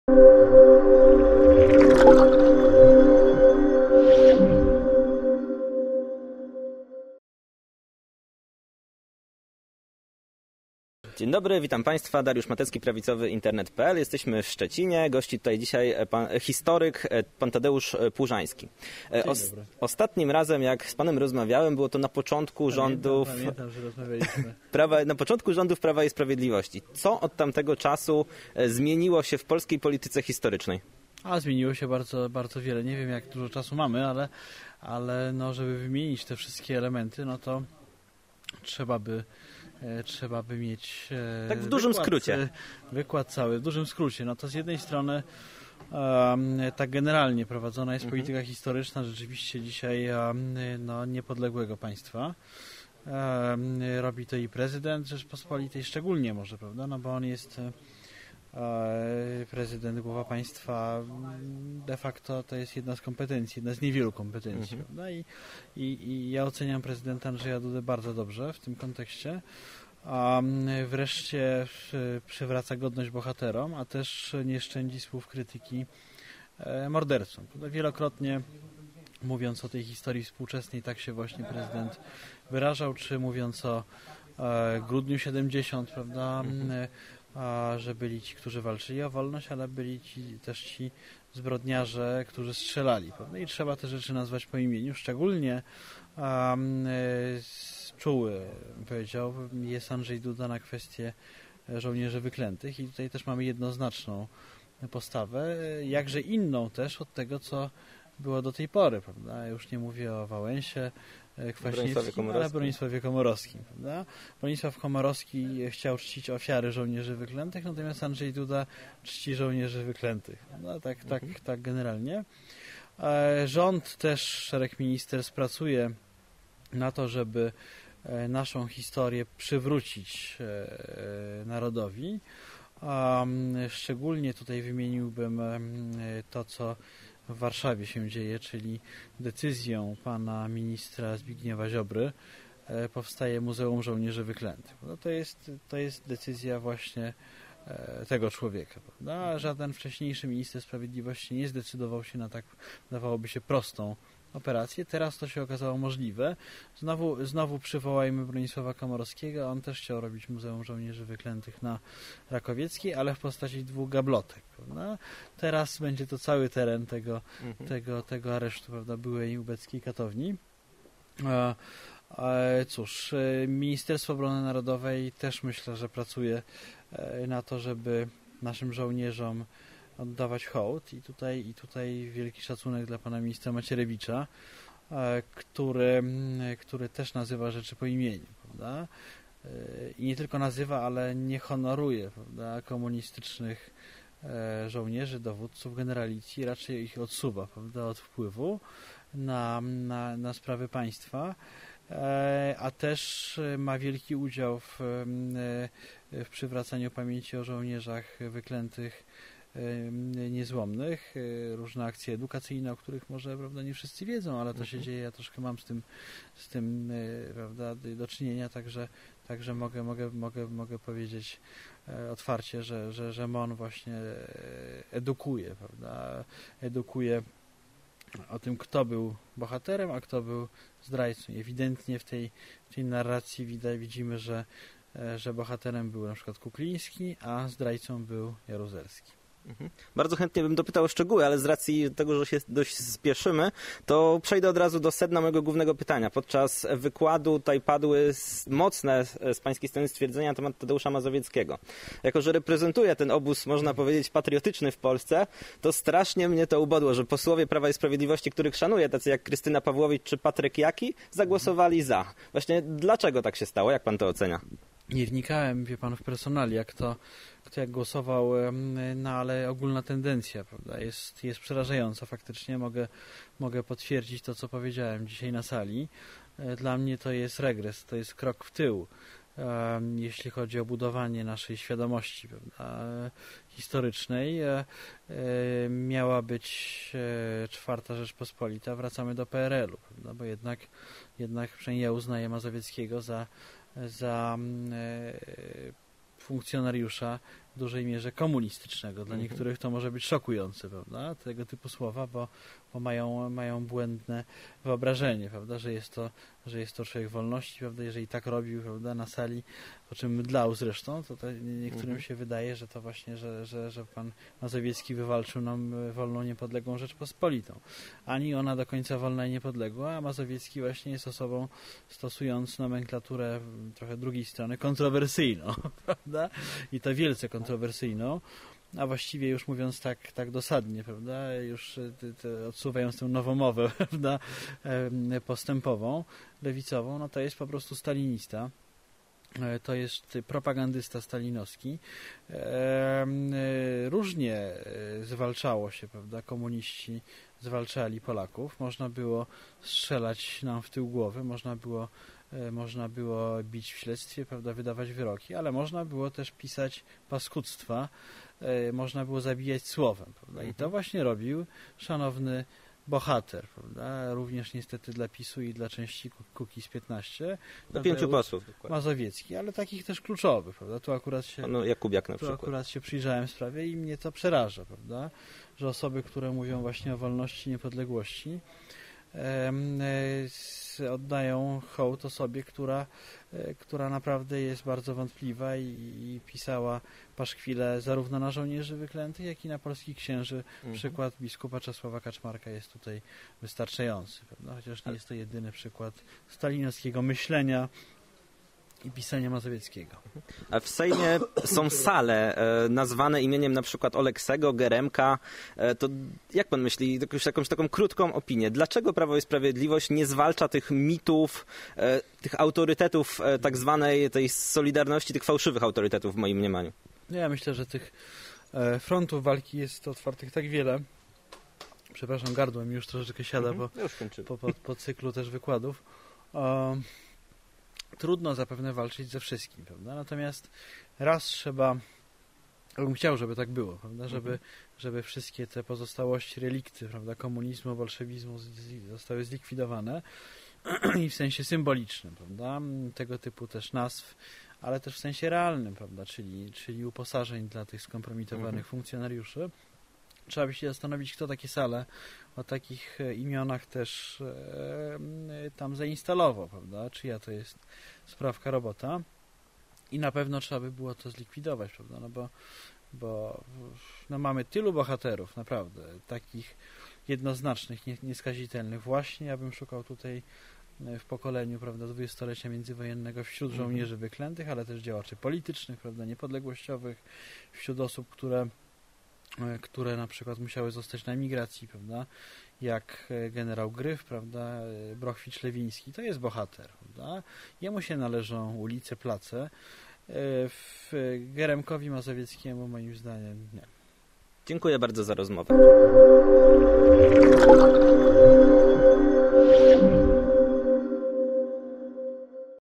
O UHONOROWANIU MAZOWIECKIEGO PRZEZ POLSKI SEJM - rozmowa z T. Płużańskim
Na wniosek Platformy Obywatelskiej i Nowoczesnej, Sejm podjął uchwałę związaną z 90. rocznicą urodzin pierwszego niekomunistycznego premiera Tadeusza Mazowieckiego. Postać polityka budzi na tyle duże kontrowersje, że część posłów Prawa i Sprawiedliwości oraz klubu Kukiz’15 opuściło salę plenarną. Nie wszyscy obecni na sali posłowie byli zgodni ws. uchwały. 39 z nich zagłosowało przeciwko upamiętnieniu polityka Unii Wolności, który w latach 50-tych XX wieku niezwykle krytycznie wypowiadał się nt. Żołnierzy Wyklętych i rządu Rzeczypospolitej na uchodźctwie.